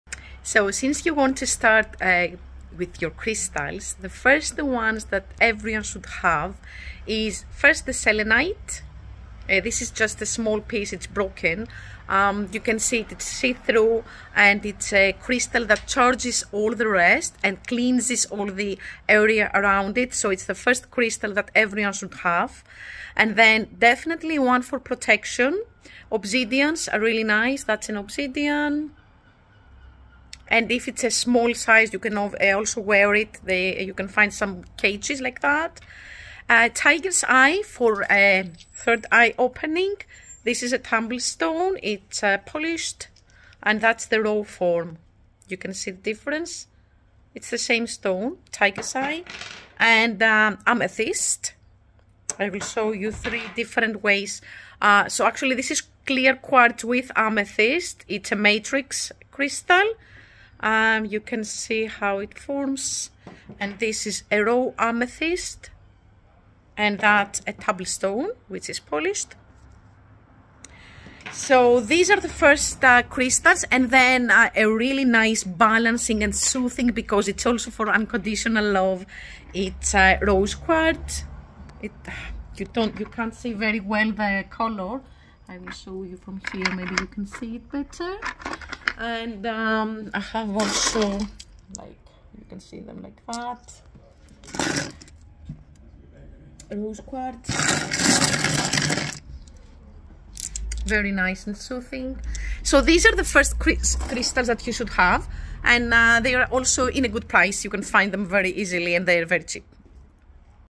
crystals for beginners
For those embarking on a journey of spiritual discovery and self-improvement, incorporating crystals into your daily practice can offer significant benefits. As you begin this path, there are a few essential crystals you might consider acquiring first, each serving a unique purpose in your spiritual toolkit.

Selenite: The selenite crystal is known for its powerful cleansing and charging abilities. It can purify the energy of your space and other crystals, removing negativity and promoting a calm, peaceful environment. Keeping selenite close ensures a constant flow of good vibes, making it a cornerstone for any crystal collection.

Amethyst: Renowned for its stunning purple hue, amethyst is much more than a pretty stone. It's a spiritual workhorse that enhances intuition and offers protection against negative energies. Amethyst can help you tap into your inner wisdom and shield you from psychic attacks, making it an invaluable ally in your spiritual growth.

Onyx: This powerful grounding stone, which is similar to the black obsidian, is essential for those seeking protection and stability. Onyx absorbs and transforms negative energy, helping to prevent the drain of personal energy and supporting emotional and physical strength in times of stress or grief. It’s a stone of resilience, offering a solid foundation on which to build your spiritual practice.

Tiger's Eye: With its striking bands of gold and brown, tiger's eye crystal is as captivating to the eye as it is beneficial for the spirit. It's particularly known for its ability to open and enhance the third eye, improving your ability to see things clearly and make decisions with wisdom and courage. It also provides protection and grounding, balancing yin-yang energies and fostering harmony.

Rose Quartz: Often called the stone of love, rose quartz promotes a deep sense of personal fulfillment and contentment. It opens the heart to all types of love - love of the self, love of family, love of friends, and romantic love. Its soothing vibrations are a balm to the emotions, helping to dispel negativity and nurture a gentle, forgiving attitude towards oneself and others.

Each of these crystals offers unique benefits that can support and enhance your spiritual journey. Whether you're seeking protection, love, clarity, or cleansing, these stones provide a strong foundation for your practices and intentions.